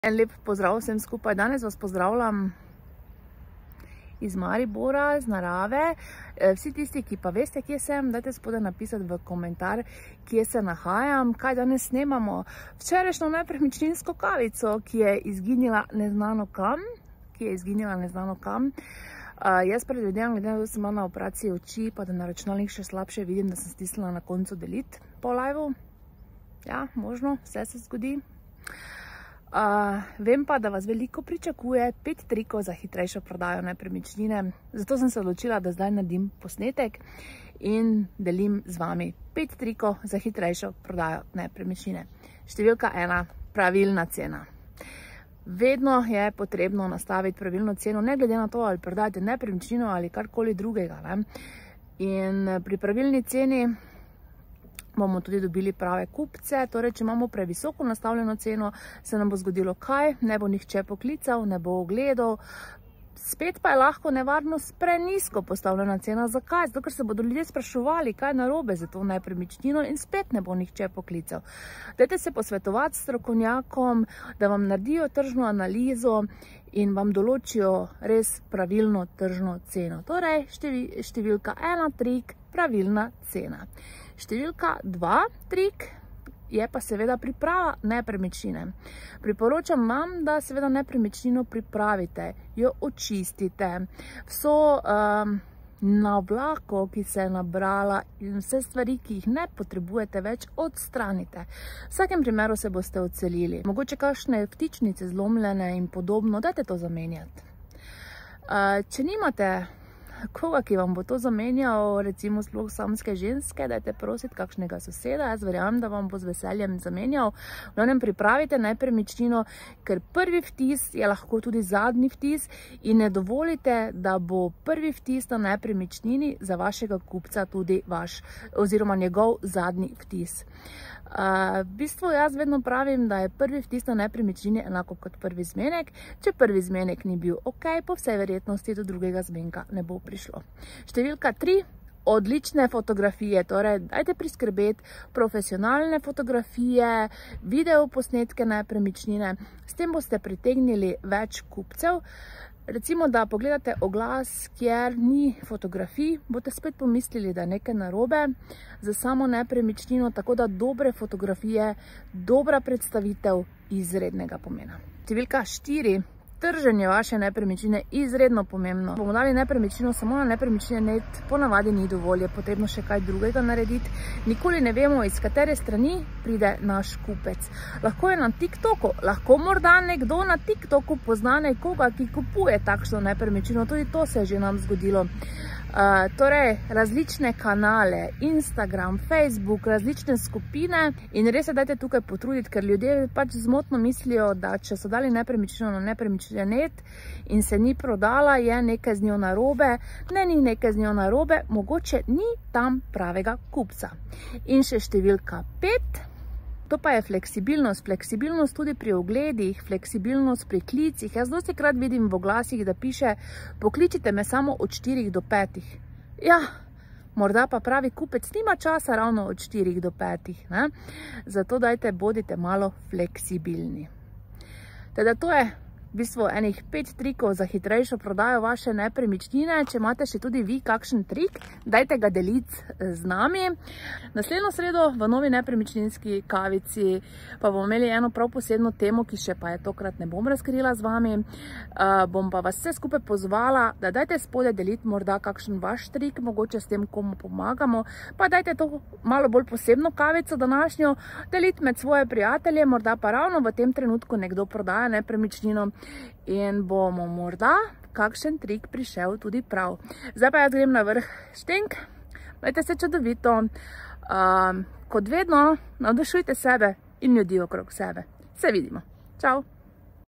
🎙️ Nepremičninska kavica I 5 trikov za hitrejšo prodajo nepremičnine
5 trikov za hitrejšo prodajo nepremičnine I 🎙️ Nepremičninska kavica

Pomembno se je zavedati, da lahko prodaja nepremičnine traja tudi nekaj časa in zahteva potrpljenje ter vztrajnost. S pravimi pristopi in strategijami pa lahko povečate možnosti za uspešno in hitrejšo prodajo.

00:00 Uvod
00:45 Trik 1: Pravilna cena
01:24 Trik 2: Priprava nepremičnine
02:22 Trik 3: Profesionalne fotografije
02:45 Trik 4: Objava na različne kanale
03:27 Trik 5: Fleksibilnost
03:44 Zaključek
Predstavljamo vam, pet trikov, ki vam lahko pomagajo pri hitrejši prodaji nepremičnine.